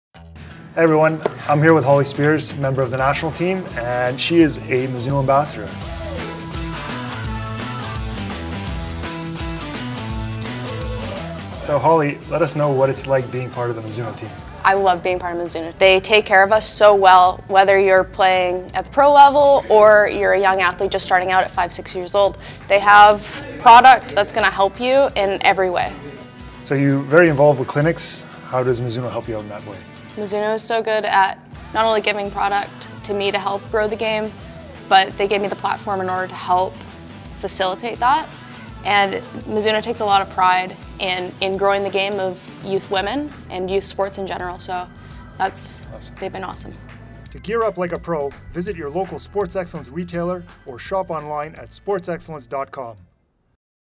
Mizuno Ambassador Holly Speers | Sports Excellence
we realize that Tokyo has been pushed 1 year, but we still want to show you the level of commitment and the daily grind these girls go through.

Day in and day out, Holly is putting in the hours to become the best. Even at the end of a long exhausting day, she put in some extra time to speak with us to discuss her journey. Give her a follow on instagram @hollyspeers10 to keep up with her journey !